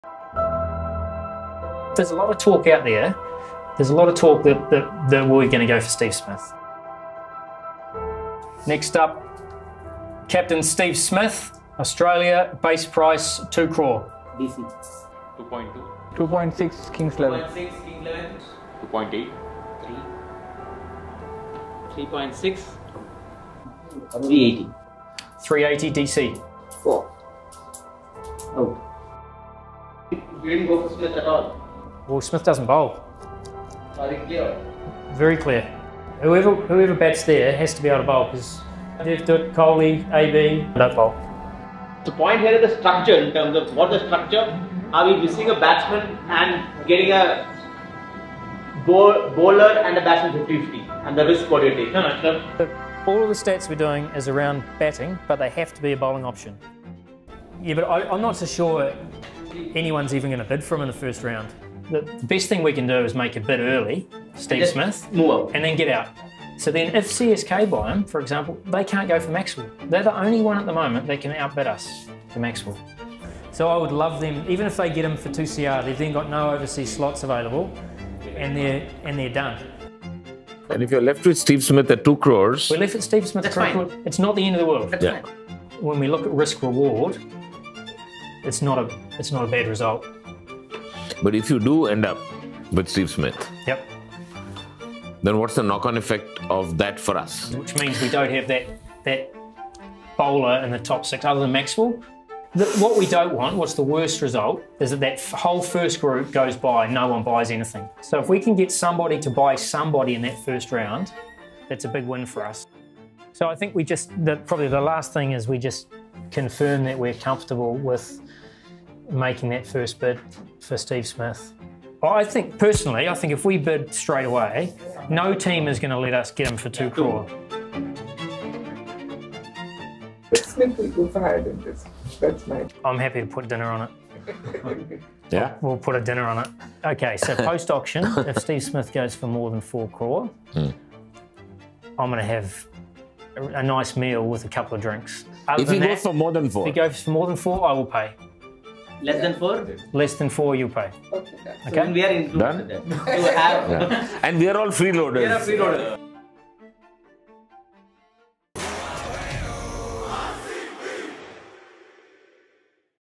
IPL Auction | The curious case of Steve Smith bid | Bold Diaries
Why did the RCB management withdraw after the first bid? Here’s the video of the planning that went behind the Steve Smith bid this IPLAuction2021.